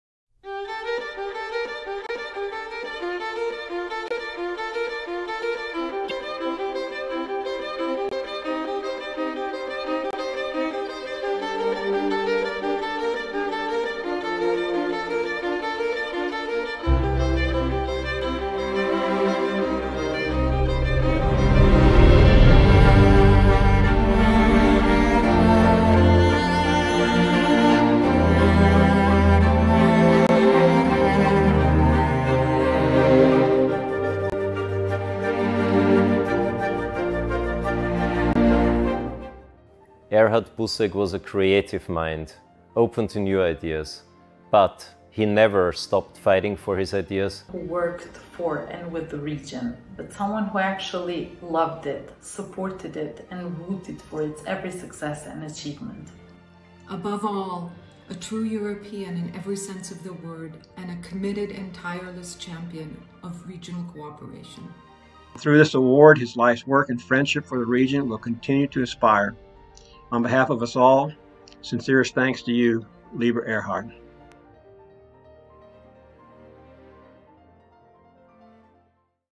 Champion of Regional Cooperation Dr. Erhard Busek
The Champion of Regional Cooperation is an annual acknowledgment to the individual or an organisation RCC considers to have contributed most to the development of regional cooperation in SEE. In 2022 the award changed its title to Champion of Regional Cooperation Dr. Erhard Busek, honouring posthumously Mr Busek who was the Special Co-ordinator of the Stability Pact for South Eastern Europe, the RCC’s predecessor, but also a staunch supporter of the Western Balkans and avid advocate of the region’s integration into the European Union. Above all, a good a loyal friend of RCC, Western Balkans and South East Europe.